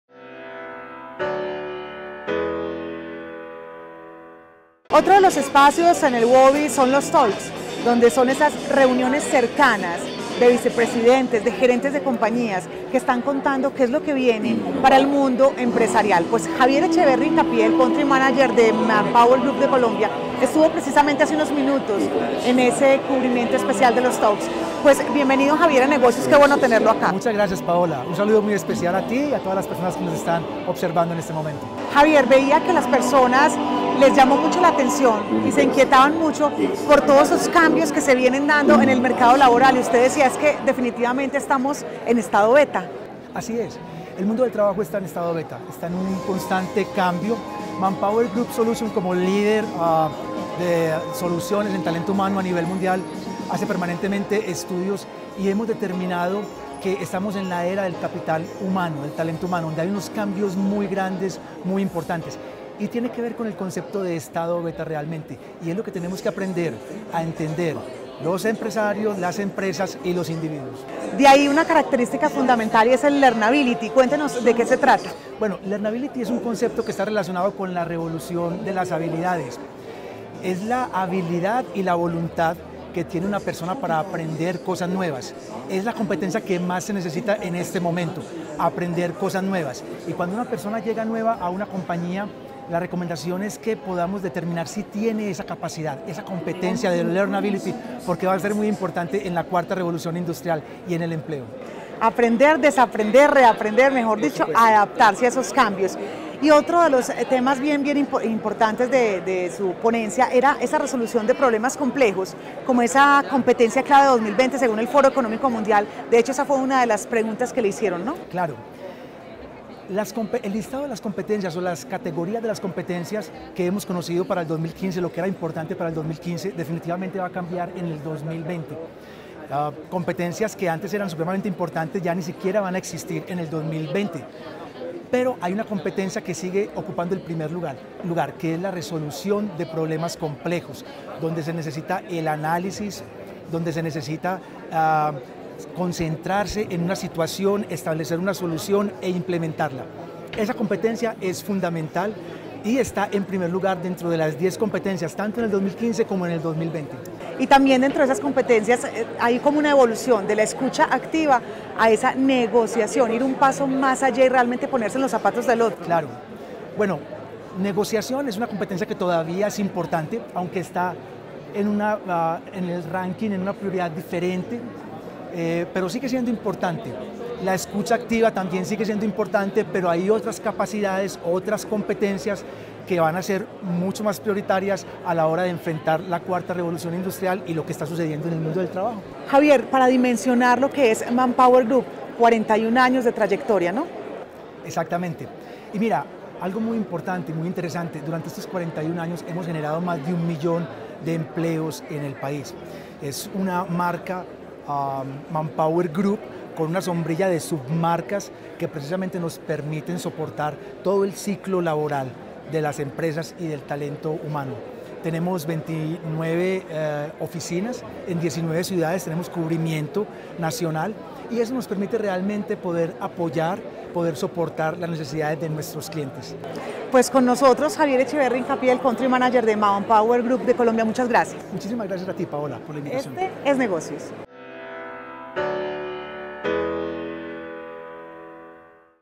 Javier Echeverri y los cambios en el mercado laboral - [Negocios en Tm] ®
La coach y empresaria internacional Paola Rueda López (@paolaruedalopez) nos habla sobre los cambios que viene experimentando el mercado laboral a nivel mundial, de la mano del Country Manager de ManpowerGroup Colombia, Javier Echeverri.